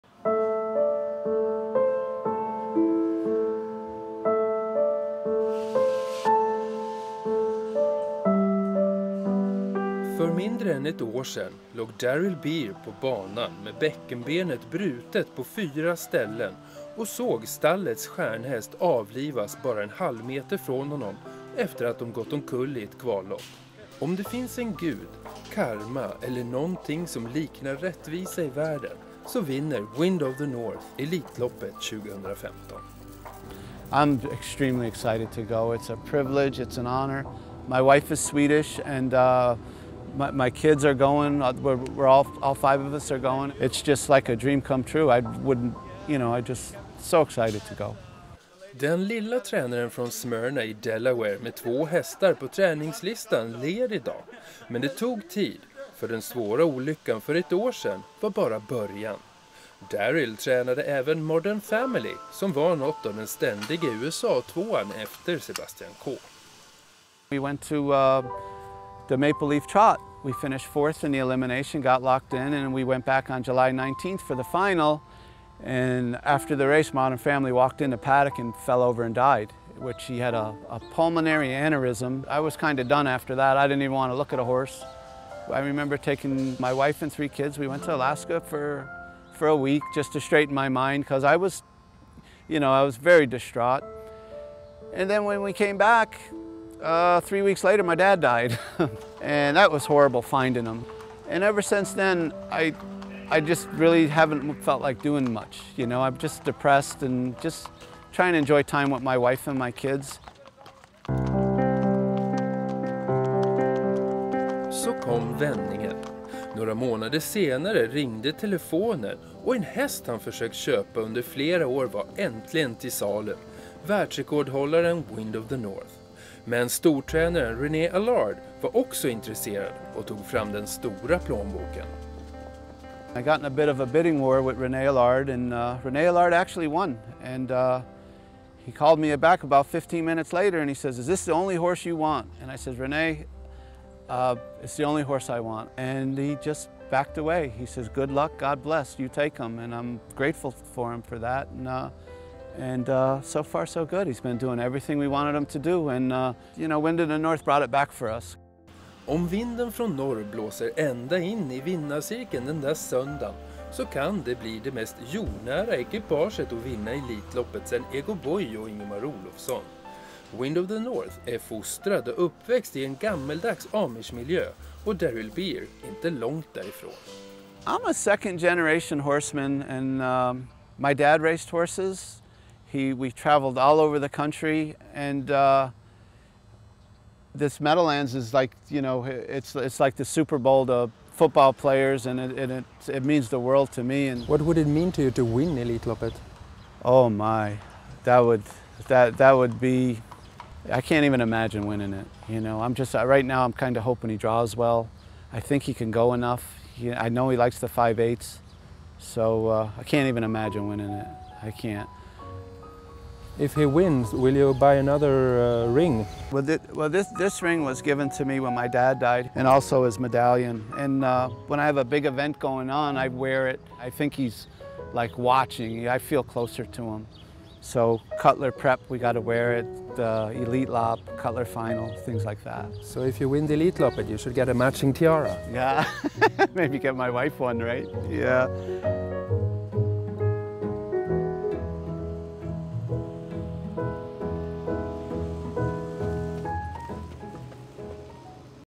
A Wind Of The North
Om det finns något som heter Gud, karma eller rättvisa är Wind Ot The North och Daryl Bier vinnare av Elitloppet 2015.
If there is something called God, Karma or justice the winner of Elitloppet 2015 would be Wind Of The North and Daryl Bier.

Reporter: Tony Ryttar
Camera & Editing: Thomas Blomqvist